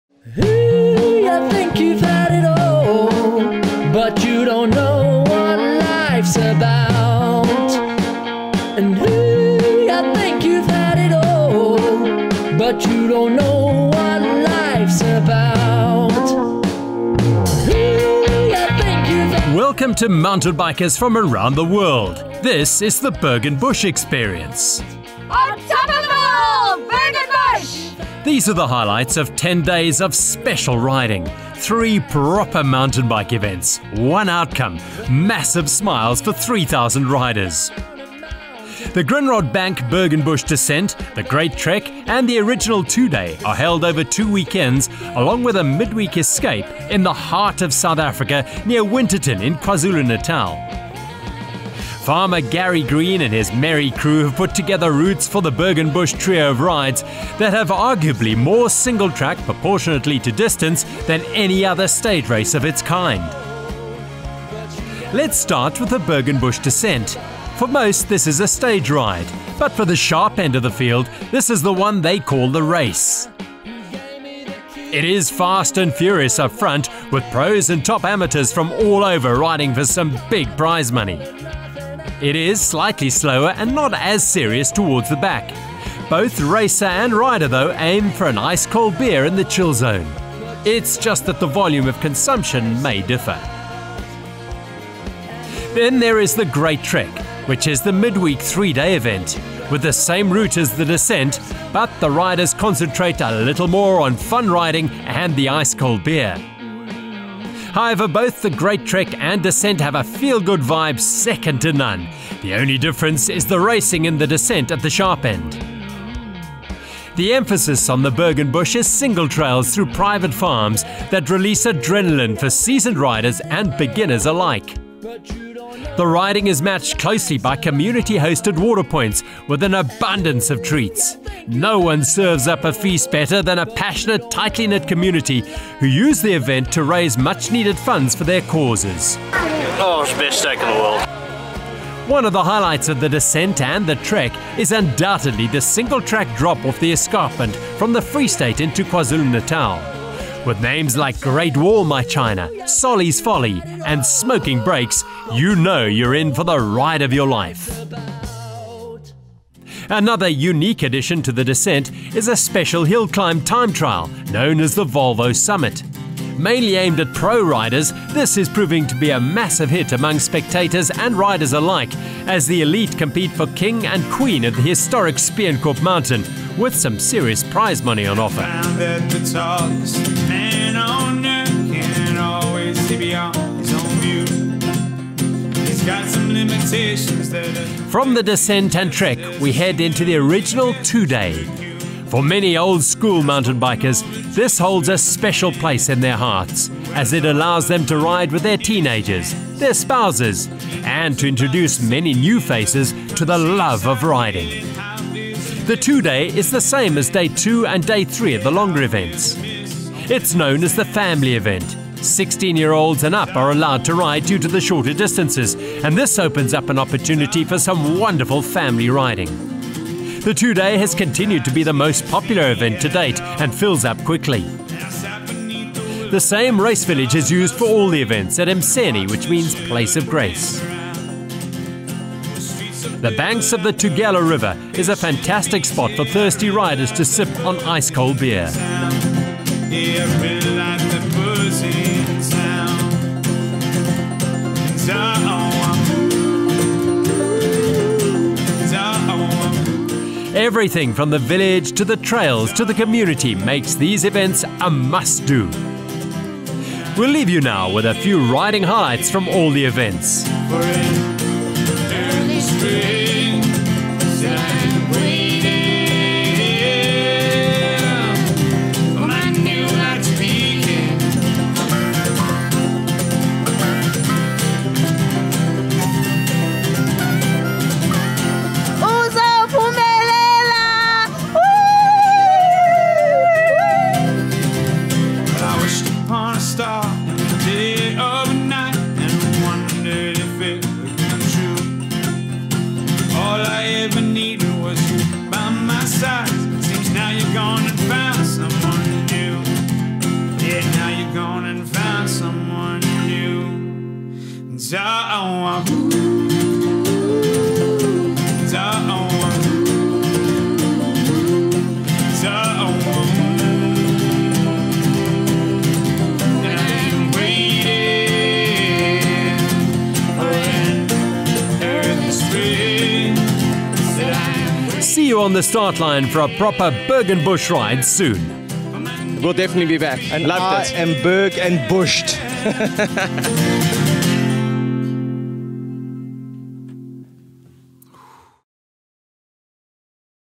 Berg & Bush 2017 Promo video
The Grindrod Bank Berg & Bush consists of three events in Winterton, KZN in South Africa. The Descent, The Great Trek, and the Two day are guaranteed to ensure you find the event to suit you - entries open 1 Feb 2017.